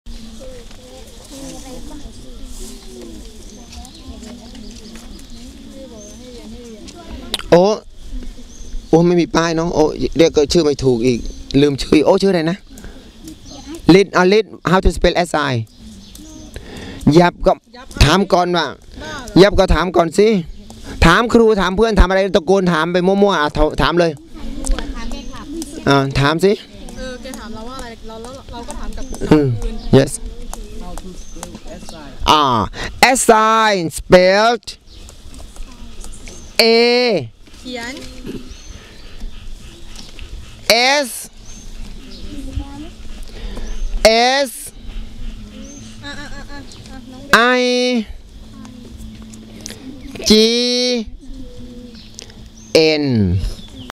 English Summer Camp 2012 (26)
Pradit Deerorb with children on English summer camp 2012 at Koke Prasart, Gratiam, Sungkha, Surin, Thailand.